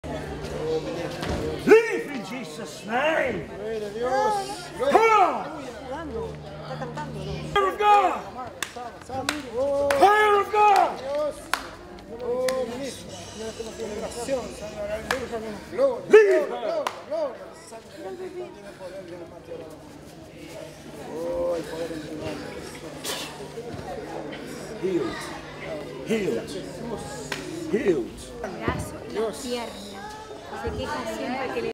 Johann van der Hoven - Delivered from demonic oppression
Set Free and delivered from a spirit of oppression that caused heart disease and constant pain down her left arm and chest.